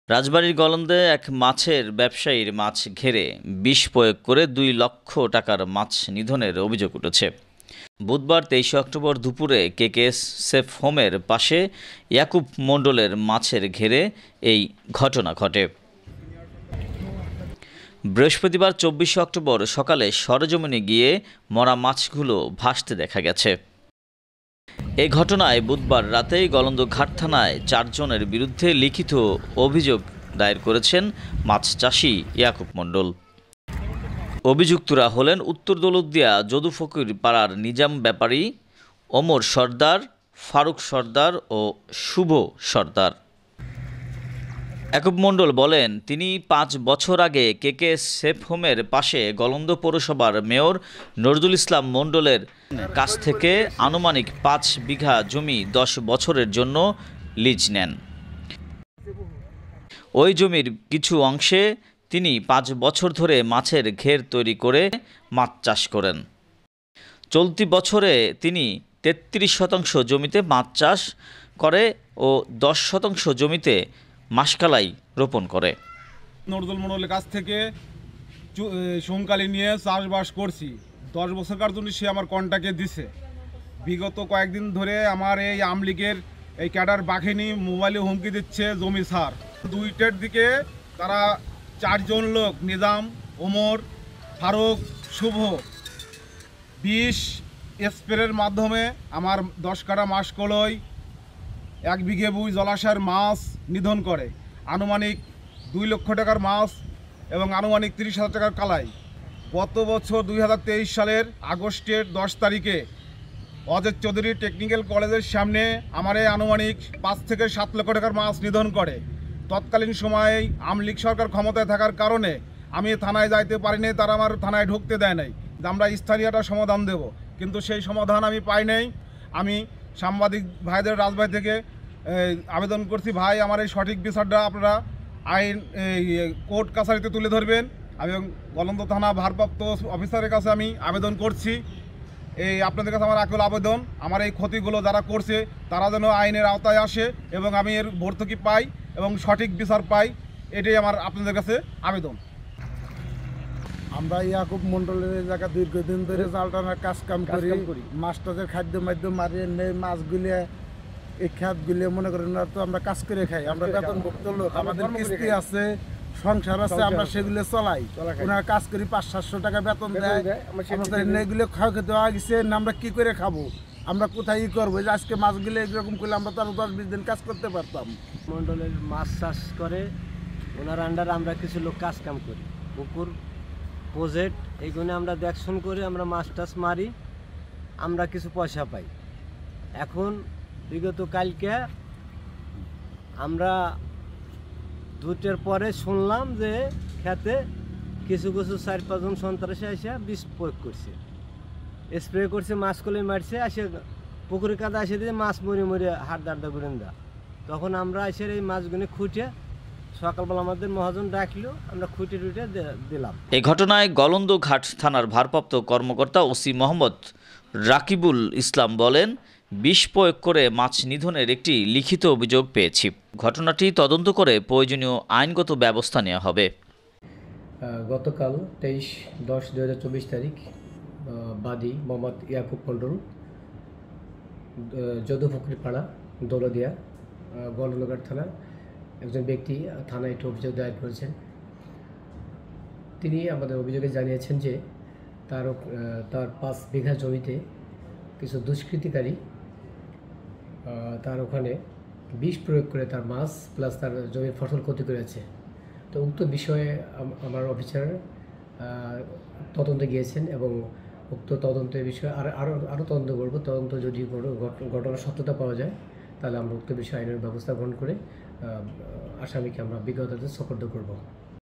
রাজবাড়ীতে বিষ দিয়ে কয়েক লাখ টাকার মাছ নিধন থানায় লিখিত অভিযোগ | Bangla News | Update News
রাজবাড়ীতে বিষ দিয়ে কয়েক লাখ টাকার মাছ নিধন থানায় লিখিত অভিযোগ  | Bangla News | Update News | Global TV News